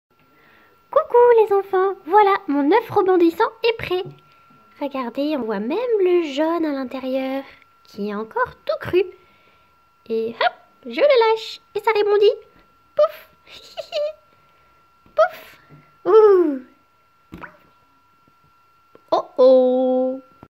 Expérience oeuf rebondissant 24h plus tard
Biscotte fait une petite bêtise!